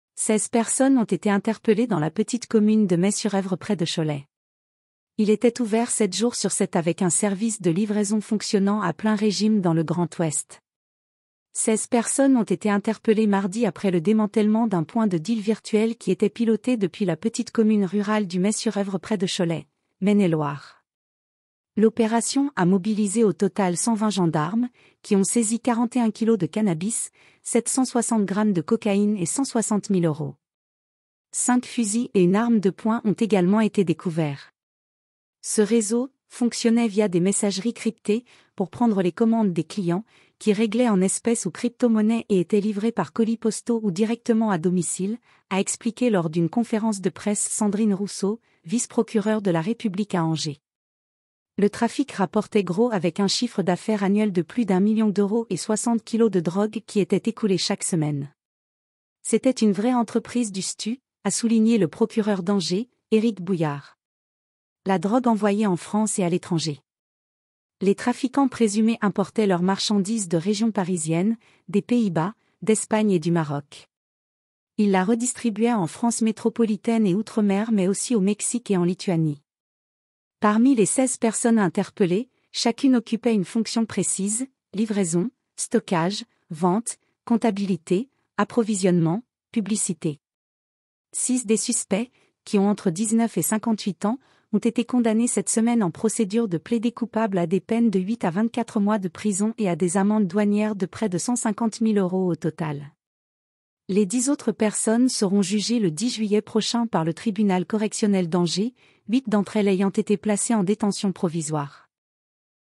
Maine-et-Loire : Un point de deal virtuel qui opérait depuis la campagne démantelé
Seize personnes ont été interpellées dans la petite commune de May-sur-Evre près de Cholet
Il était ouvert sept jours sur sept avec un service de livraison fonctionnant à plein régime dans le Grand Ouest. Seize personnes ont été interpellées mardi après le démantèlement d’un point de deal virtuel qui était piloté depuis la petite commune rurale du May-sur-Evre près de Cholet (Maine-et-Loire). L’opération a mobilisé au total 120 gendarmes, qui ont saisi 41 kilos de cannabis, 760 grammes de cocaïne et 160.000 euros. Cinq fusils et une arme de poing ont également été découverts.
Ce réseau « fonctionnait via des messageries cryptées » pour prendre les commandes des clients, qui réglaient en espèces ou cryptomonnaies et étaient livrés par colis postaux ou directement à domicile, a expliqué lors d’une conférence de presse Sandrine Rousseau, vice-procureure de la République à Angers. Le trafic rapportait gros avec un chiffre d’affaires annuel de plus d’un million d’euros et 60 kilos de drogue qui étaient écoulés chaque semaine. « C’était une vraie entreprise du stup », a souligné le procureur d’Angers, Éric Bouillard.
La drogue envoyée en France et à l’étranger
Les trafiquants présumés importaient leur marchandise de région parisienne, des Pays-Bas, d’Espagne et du Maroc. Ils la redistribuaient en France métropolitaine et outre-mer mais aussi au Mexique et en Lituanie. Parmi les seize personnes interpellées, chacune occupait une fonction précise : livraison, stockage, vente, comptabilité, approvisionnement, publicité…
Six des suspects, qui ont entre 19 et 58 ans, ont été condamnés cette semaine en procédure de plaider-coupable à des peines de huit à vingt-quatre mois de prison et à des amendes douanières de près de 150.000 euros au total. Les dix autres personnes seront jugées le 10 juillet prochain par le tribunal correctionnel d’Angers, huit d’entre elles ayant été placées en détention provisoire.